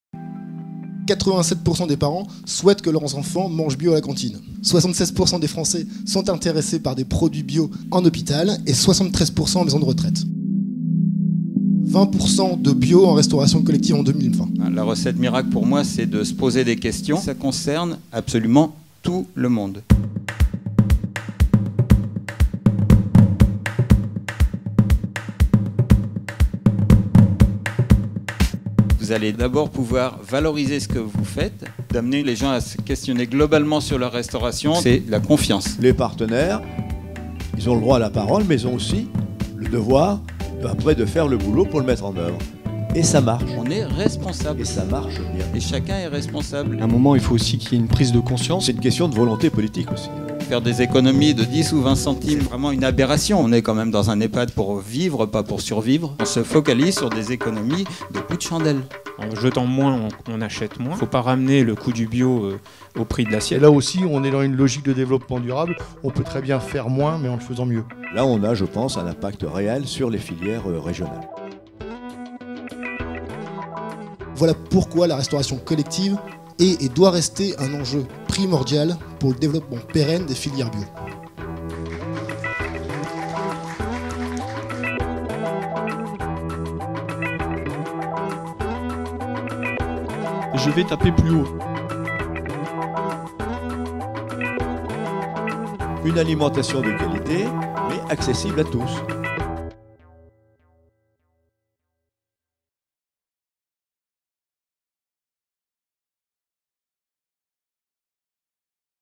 Coopérabio, c'est quoi?
Depuis 2015, A PRO BIO organise La journée de la restauration collective des Hauts-de-France. Découvrez les sujets abordés en 2016.